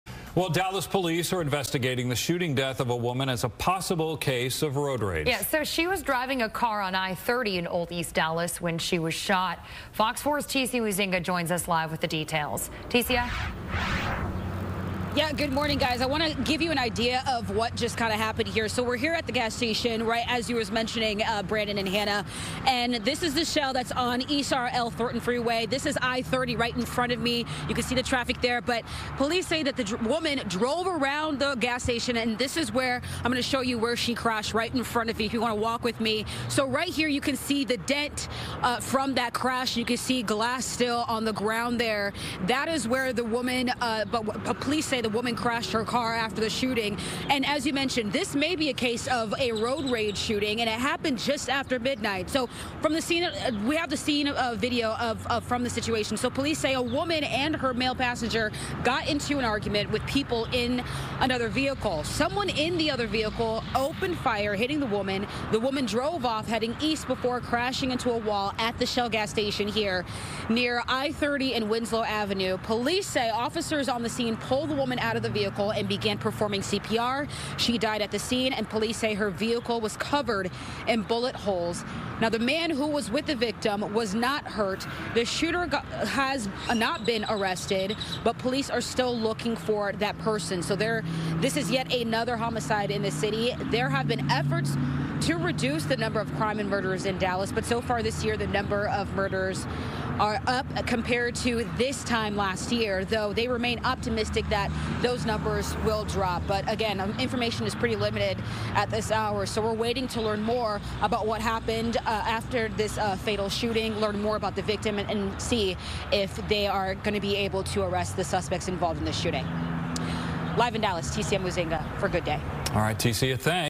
Woman shot to death while driving in Dallas
A woman was fatally shot while driving a car in Dallas overnight. Police think she may have been a victim of road rage.